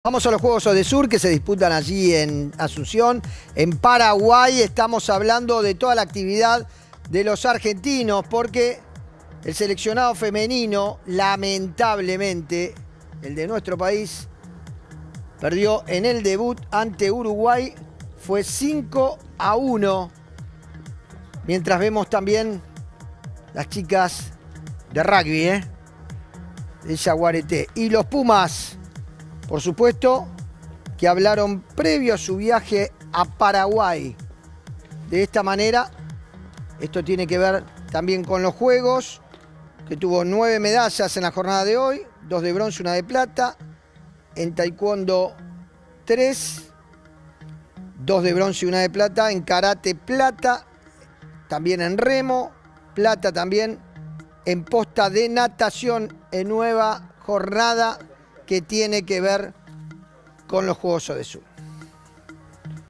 Juegos Odesur 2022: el papel de la delegación argentina
Luego de los tres primeros días de competencia, la delegación argentina se colgó 56 medallas en los Juegos Suramericanos Asunción 2022: 9 doradas, 21 plateadas y 26 de bronce. Este 5 de octubre, cuarto día de competencia, los atletas nacionales siguieron ampliando el medallero.